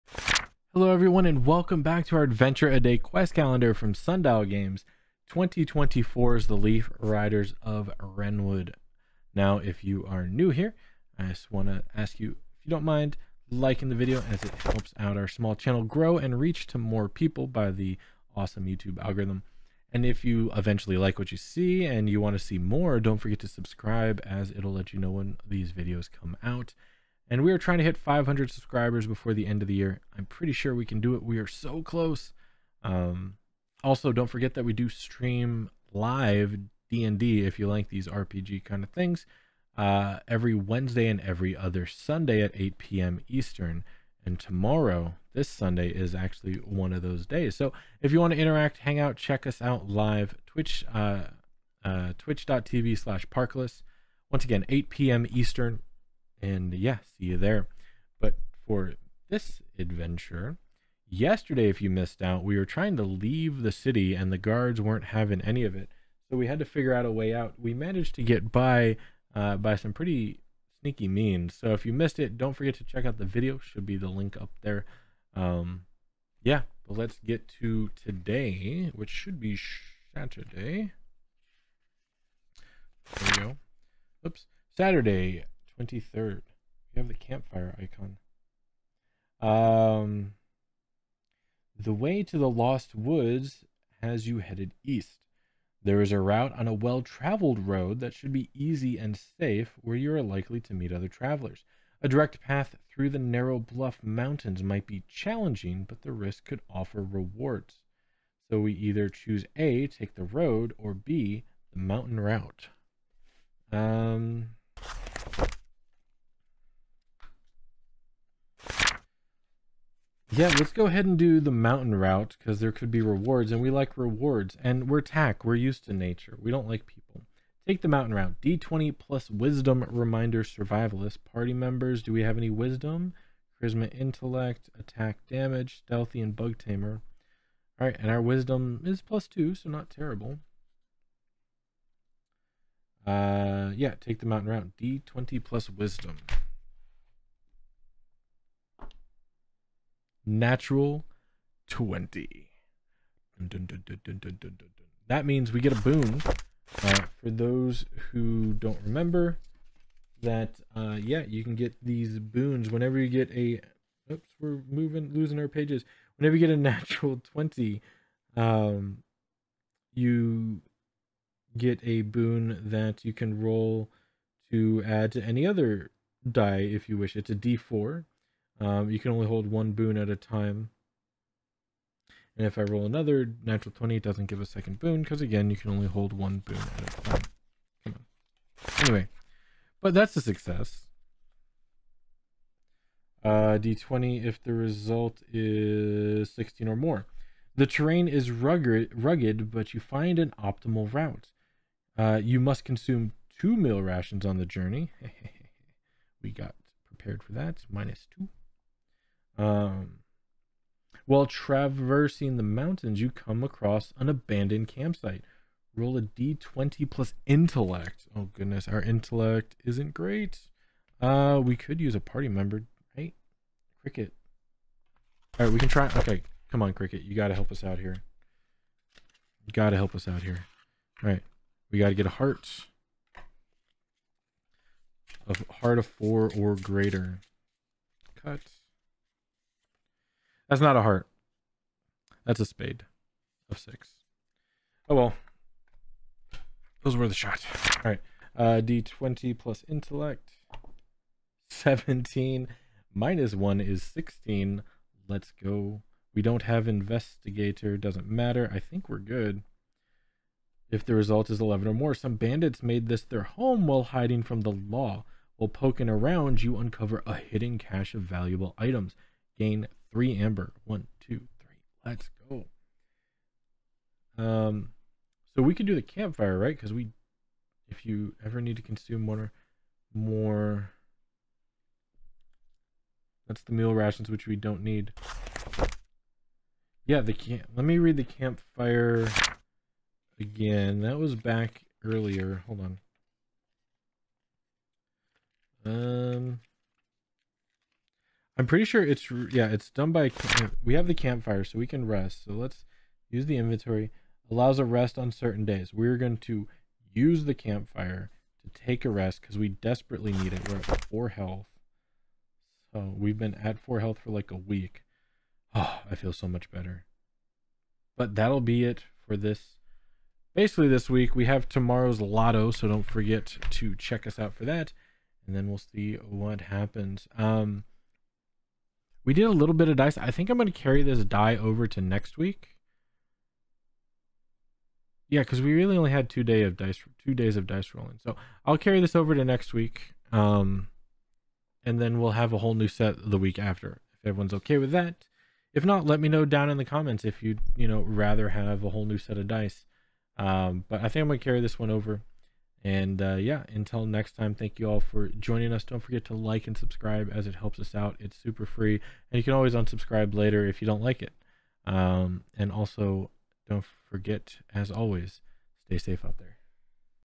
The Road To The Lost Woods Is ROUGH! - Day 83
We stealth past  the Guards at Crownspire Castle and make our way towards the Lost Woods! The adventure continues with TAK REDWIND .

From the wonderful people at Sundial Games comes another great tail! Follow Tak and the gang from The Burrows on this Adventure a day D&D Style calendar.

Chapters:
0:00 Intro
0:30 Yesterday's Adventure
1:00 Today's Adventure